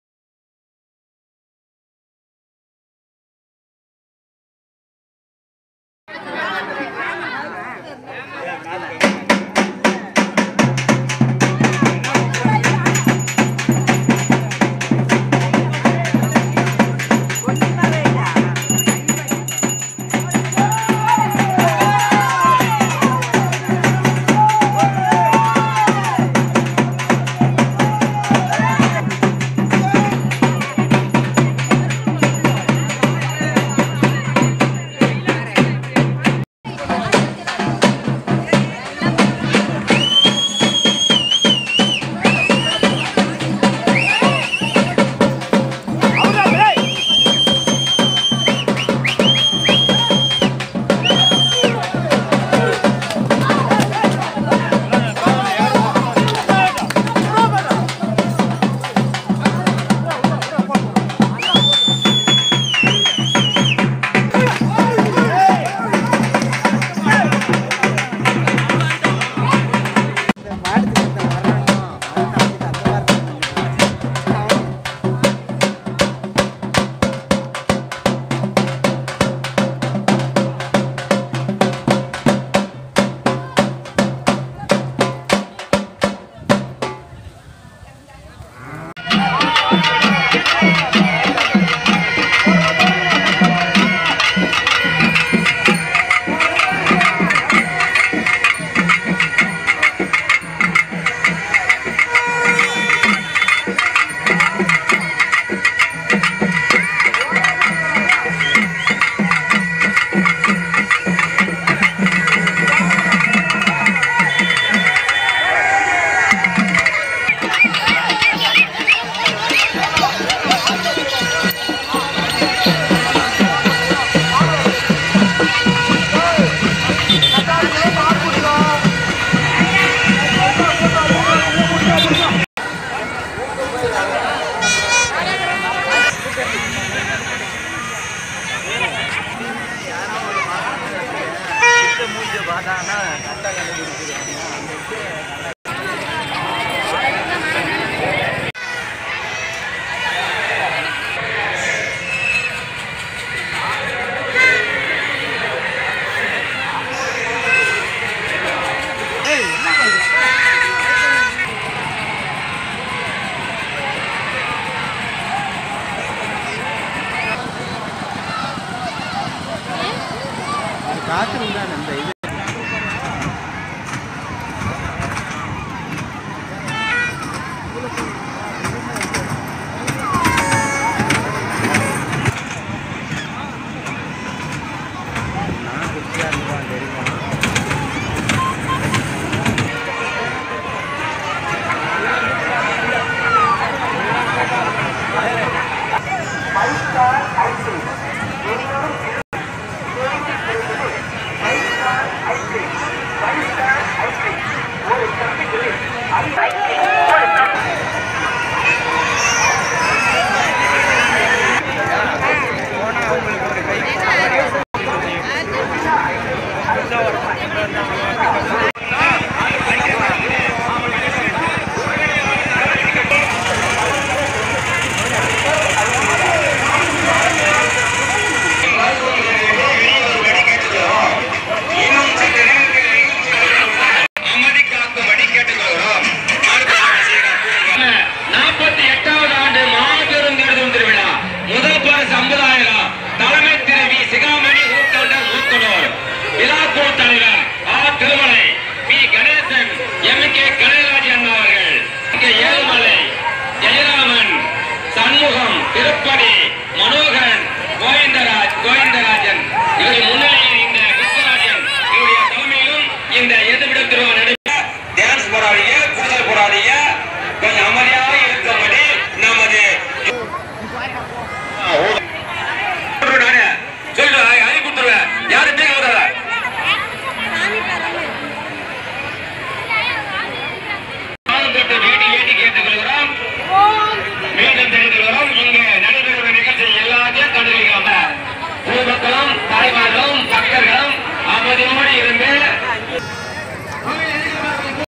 Pongal Celebration in Village || Tamilnadu Pongal Festival
Hi friends
             Tamilnadu traditional festival of  Pongal Celebration  ,if you like this Video please share and your comments.